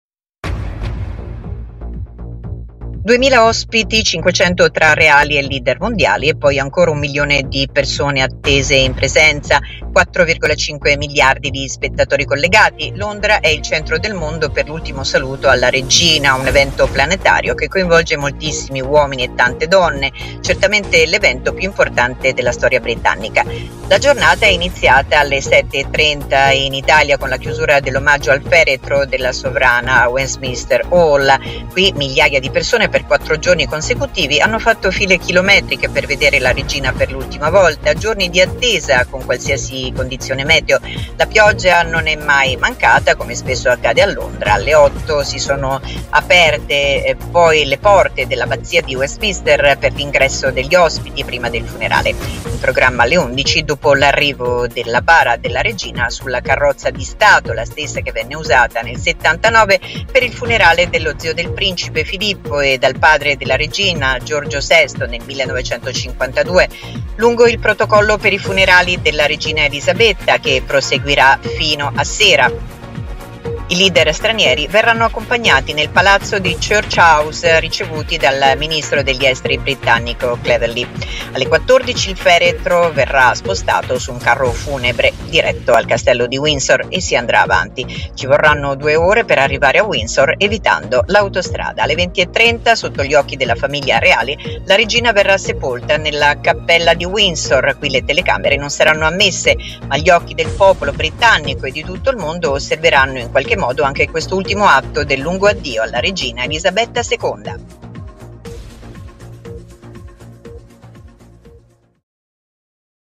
Londra, il lungo addio alla Regina Elisabetta II. È l’evento più imponente della storia britannica
Duemila ospiti. Cinquecento tra reali e leader mondiali.